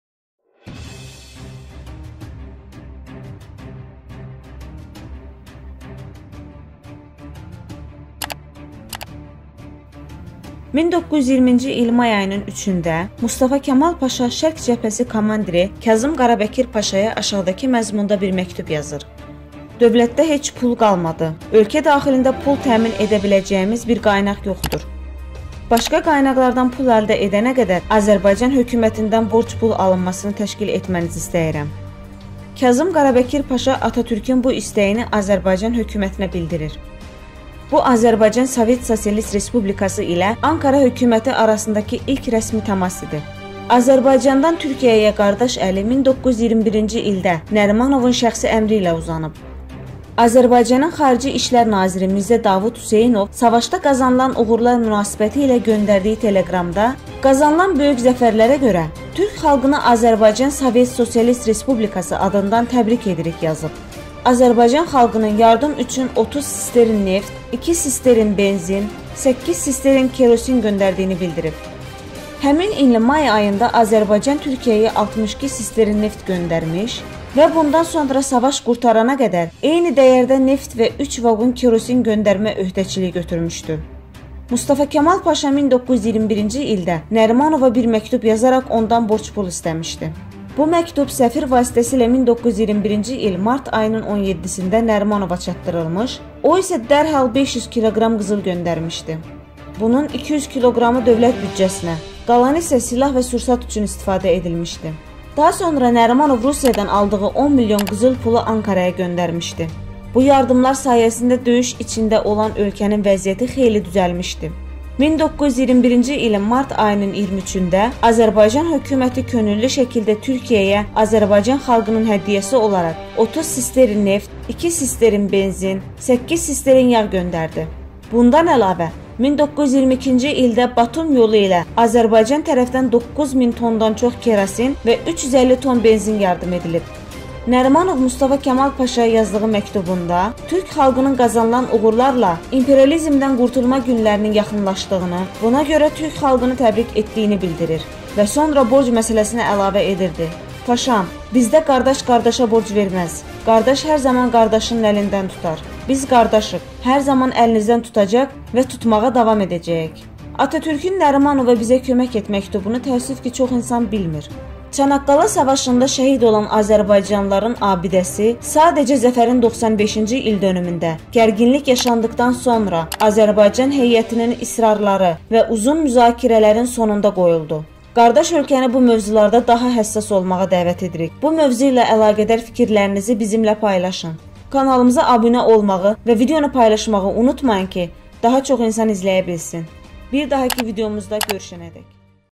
Mustafa Kamal Atatürkün Nəriman Nərimanova Yazdığı O Məktub. Nəriman Nərimanovun Cavabı ?
1920-ci il may ayının 3-də Mustafa Kamal Paşa Şərq Cəbhəsi komandiri Kazım Qarabəkir Paşaya aşağıdakı məzmunda bir məktub yazır: “Dövlətdə heç pul qalmadı. Ölkə daxilində pul təmin edə biləcəyimiz bir qaynaq yoxdur. Başqa qaynaqlardan pul əldə edənə qədər, Azərbaycan Hökumətindən borc pul alınmasını təşkil etmənizi istəyirəm”.
Bizə dəstək üçün kanala abunə olmağı, bəyənməyi və rəy yazmağı unutmayın. Fərqli səslə qarşınızdayıq..Bəyəndiysəniz butuna basmağı unutmayın :)